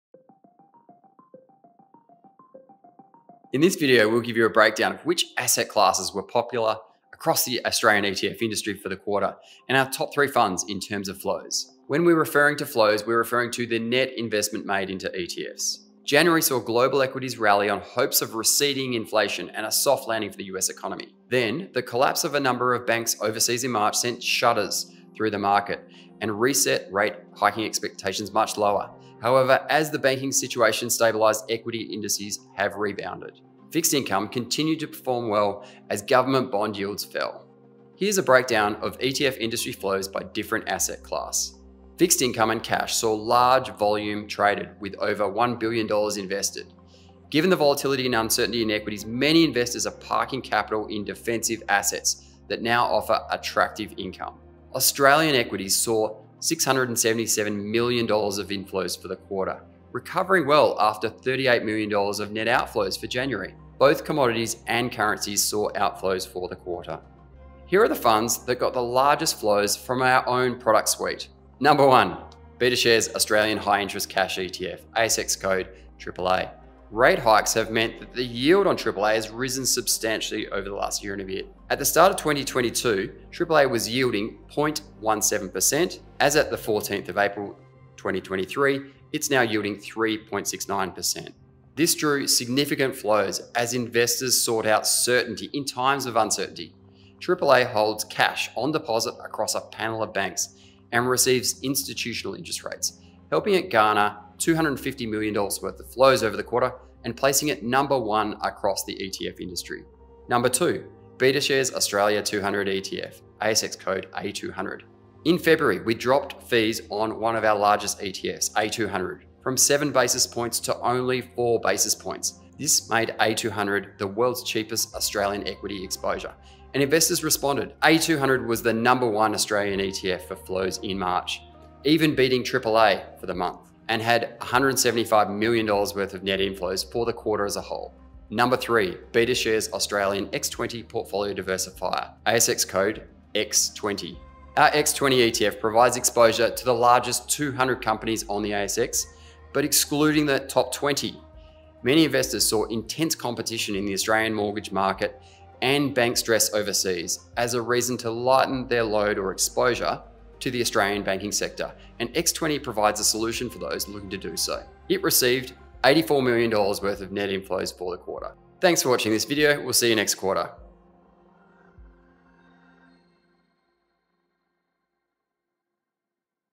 Our top 3 ETFs for Q1 2023
In this video, Cam reviews ETF flows in Australia for the first quarter of 2023, and discusses the top three Betashares ETFs for the quarter.